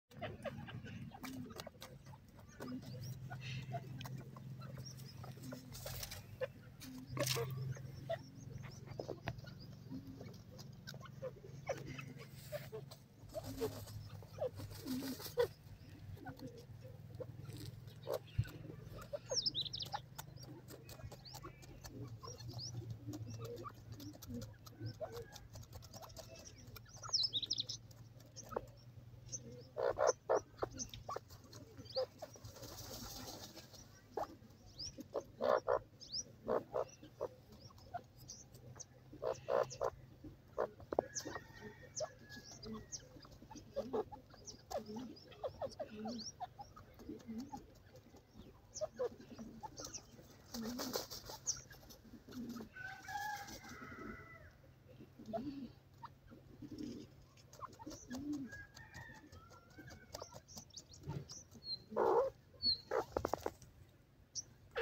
bhuray teetar apni ghur bana rahy, teetar madi tiar ha andon k liy
bhuray teetar apni ghur bana rahy, teetar madi tiar ha andon k liyWelcome to our channel, where we offer you a brief look into the intriguing universe of teetars. In this endearing video, we follow the excursion of our Bhuray Teetar pair as they persistently build their home, while the Madi Teetar sets herself up for the impending errand of laying eggs
Watch in wonderment as the Bhuray Teetars carefully accumulate twigs, leaves, and different materials to design their comfortable dwelling place
 Their collaboration and commitment to settle building feature the amazing impulses of these avian engineers
In the interim, the Madi Teetar is occupied with making herself agreeable and preparing her body for the up and coming appearance of eggs
 Her mindful way of behaving and maternal senses are a demonstration of the miracles of nature's pattern of life